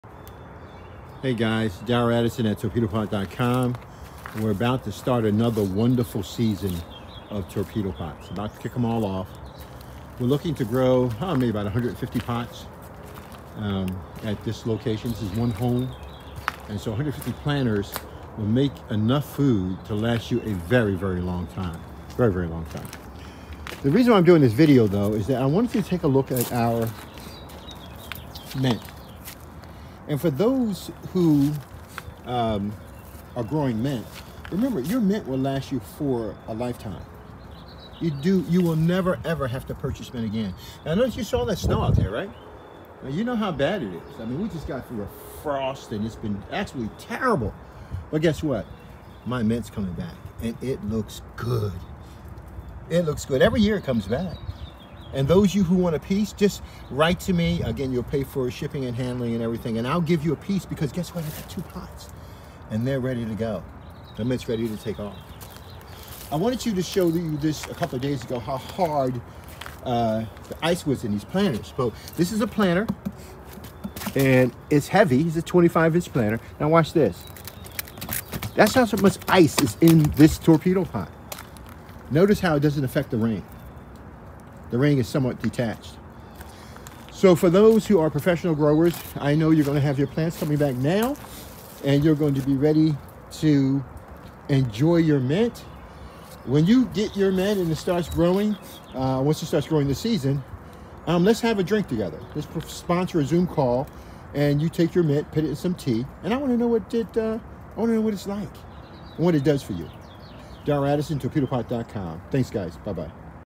My mint plants are almost ready for harvest
These mint plants come back every year on their own. We’ve been growing them for about three years now. We cut little sprigs off and give them to our neighbors and our friends. The great thing about this meant is that you can grow it inside and outside. The outside light provides a full spectrum for the plant you truly mature and grow.